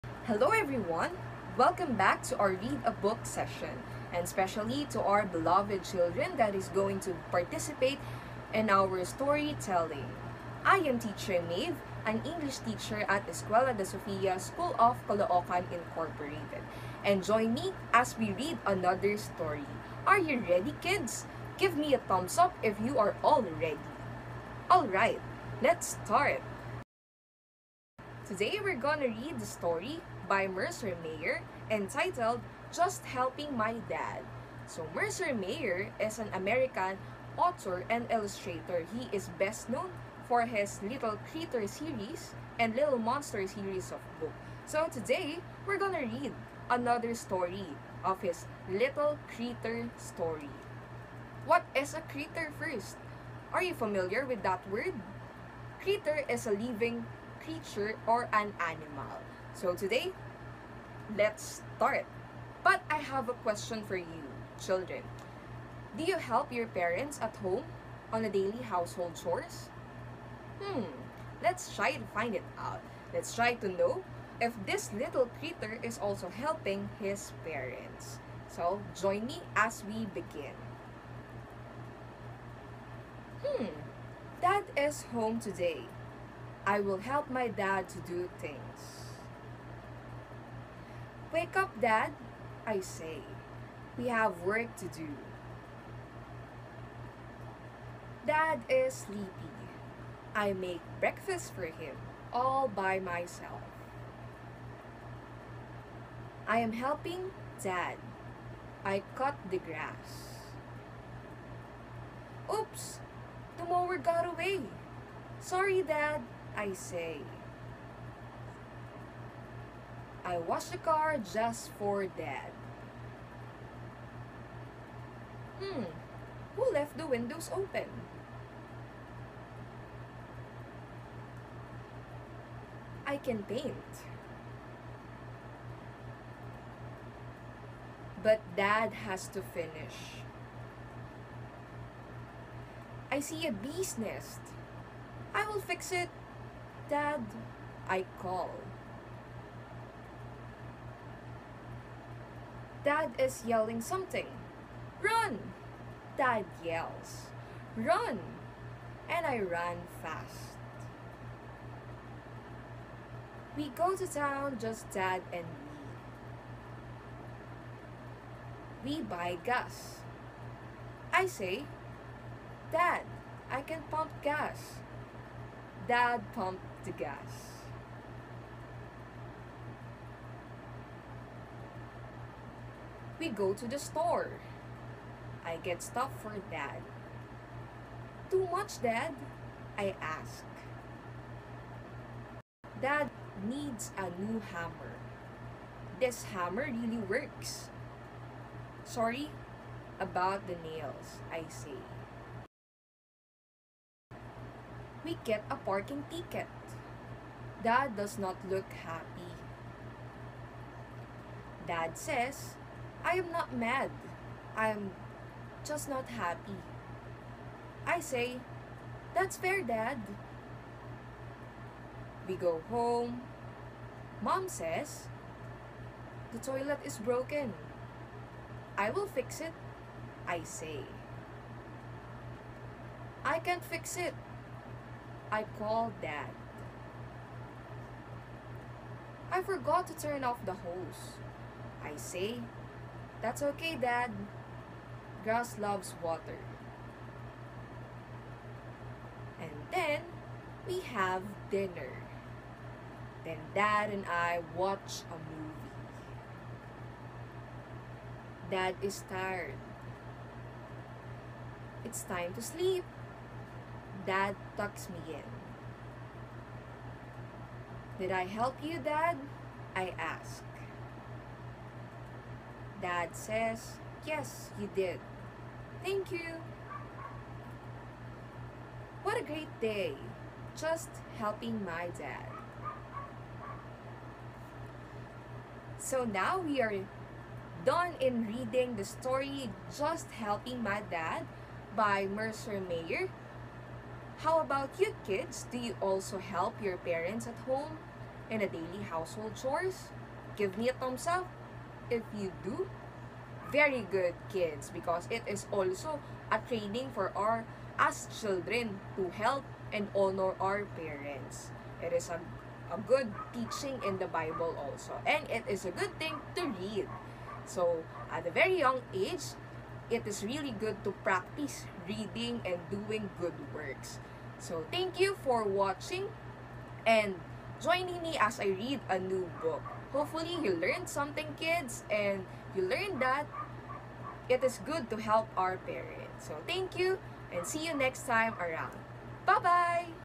JUST HELPING MY DAD by Mercer Mayer - READ ALOUD with Teacher Mave
Join Mercer Mayer’s classic and beloved character, Little Critter®, as he spends the whole day with his dad in this My First I Can Read book!
From getting rid of a beehive to working in the yard and going to the store, Little Critter wants to be the world’s best helper. Things might not always go as planned, but that doesn't stop Little Critter from trying his hardest!
Little Critter: Just Helping My Dad is a My First I Can Read book, which means it’s perfect for shared reading with young children..

From the Back Cover

Little Critter® is going to help his dad all day long! Things might not always go as planned, but that doesn't stop Little Critter from trying his hardest!

Mercer Mayer is the creator of the enduringly popular Little Critter books. Drawing from his own childhood adventures for inspiration, Mayer has created a beloved and classic character in Little Critter.